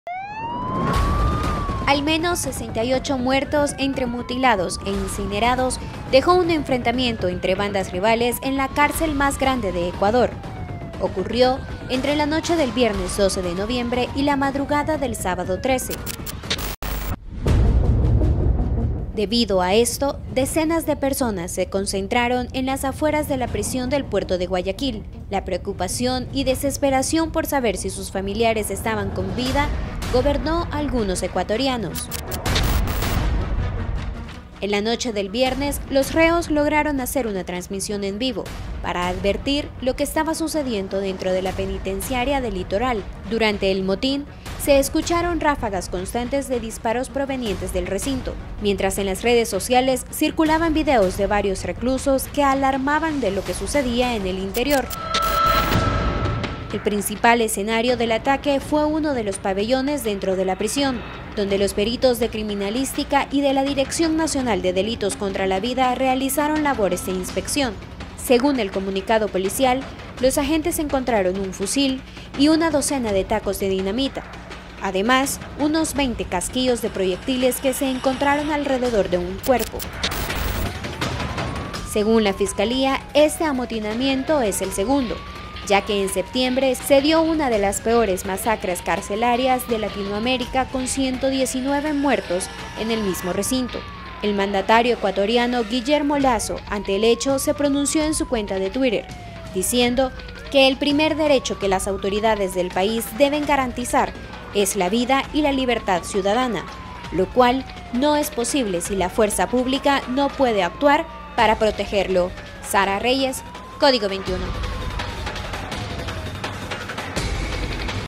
Una trifulca entre bandas rivales en Penal de Ecuador dejó al menos 68 muertos
Una trifulca entre bandas rivales en el Centro Penal más grande de Ecuador dejó al menos 68 muertos.